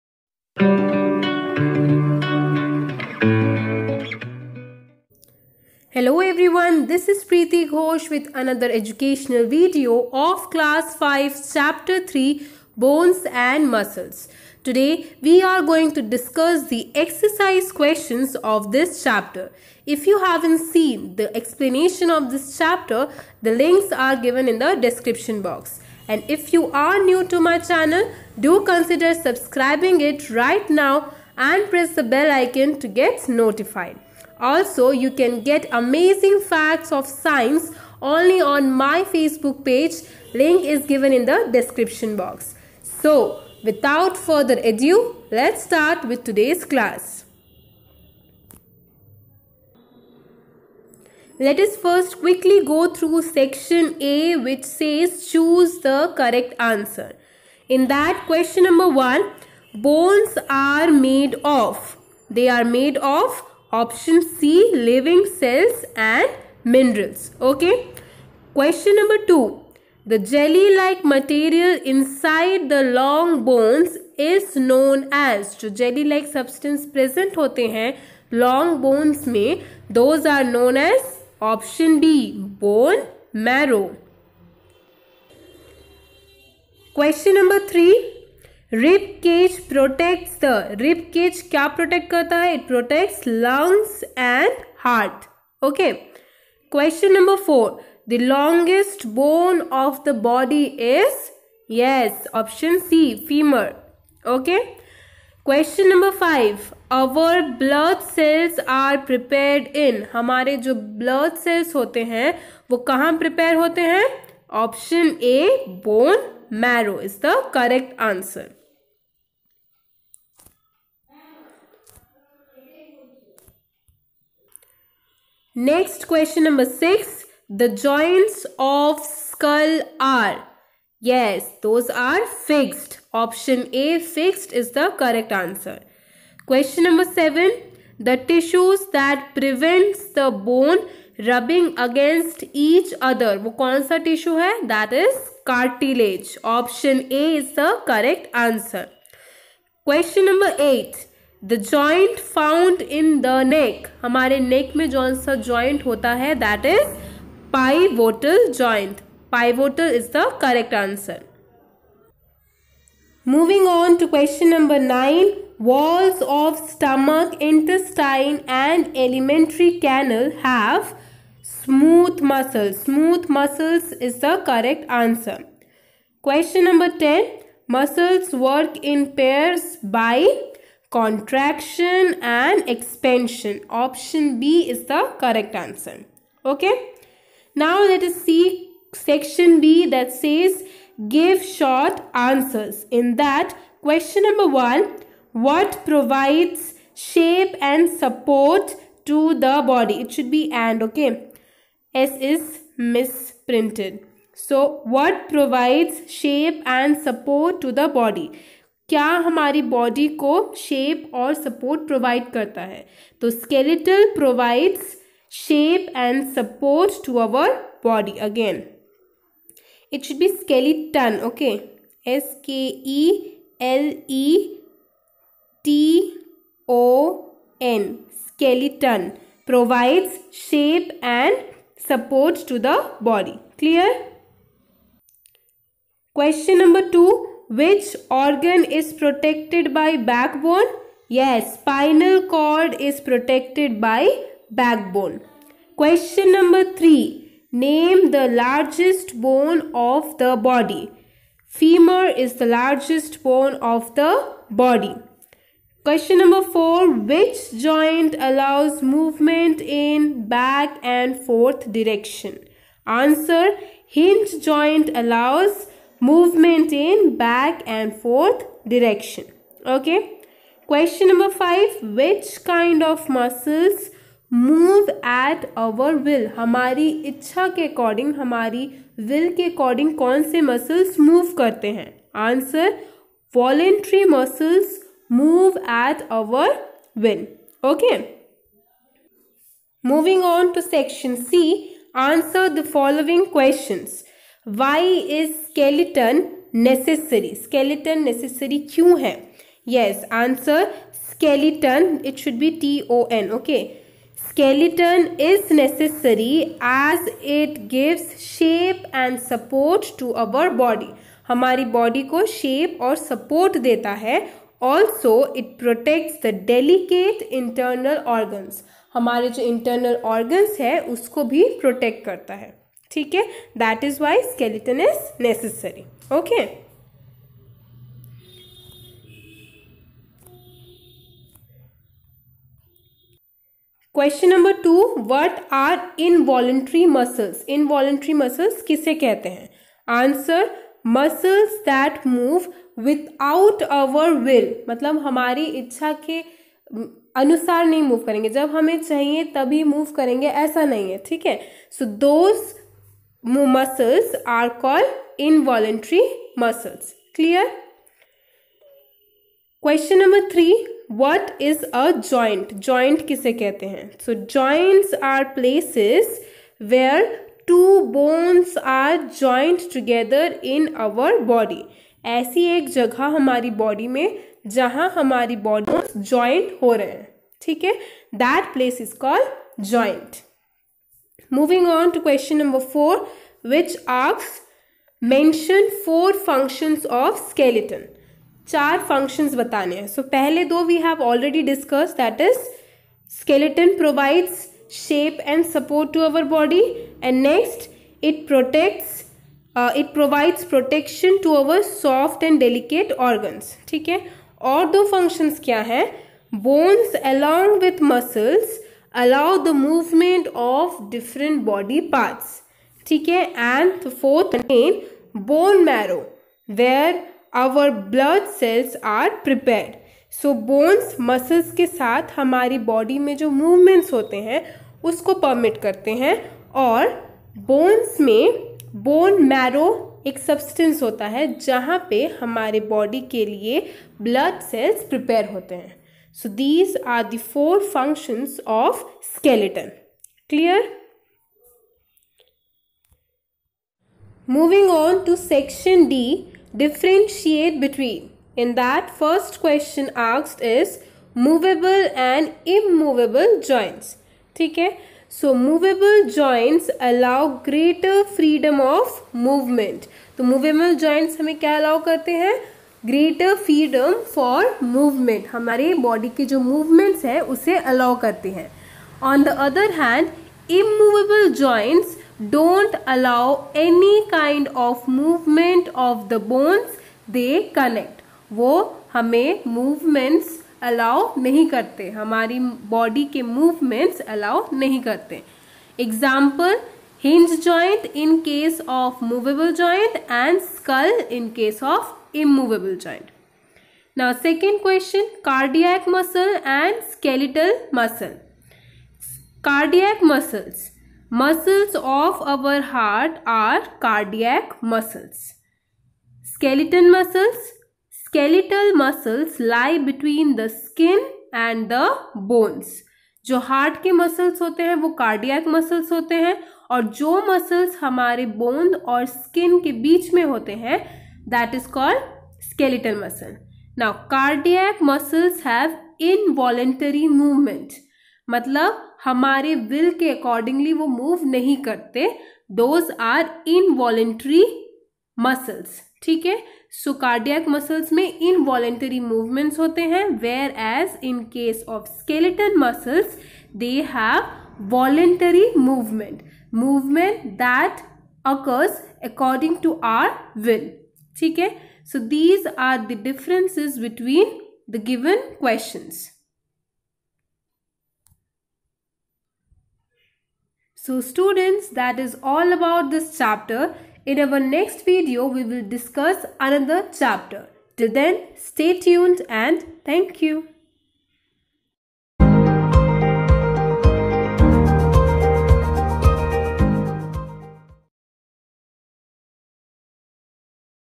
Class 5 Science Chapter 3 Exercise || CBSE Board || Prachi || Bones And Muscles (Part 4)
This is an educational video.
Class 5 Science Chapter 3
Chapter Name : Bones And Muscles (Part 4)
CBSE Board
Prachi Publication

I upload videos regarding easy explanation of CBSE based textbooks for different classes.